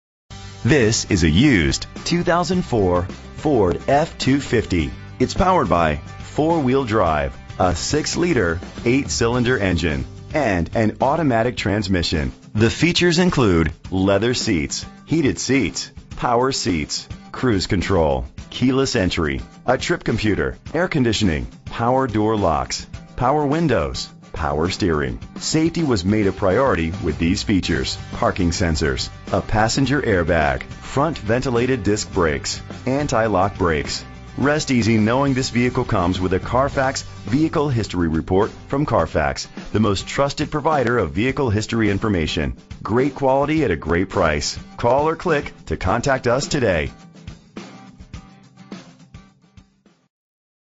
2004 Ford F-250 - Pensacola FL
Year: 2004
Make: Ford
Model: F-250
Trim: Lariat
Engine: 6.0 liter 8 cylinder 32 valve
Transmission: Automatic
Color: Medium Wedgewood Blue Metallic
Mileage: 106639
Address:
230 Beverly Parkway